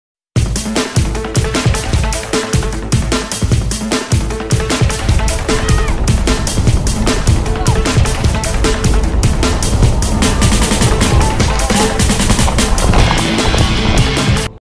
Bag Jump
Practising my jumping on the SevenSevenSports bag...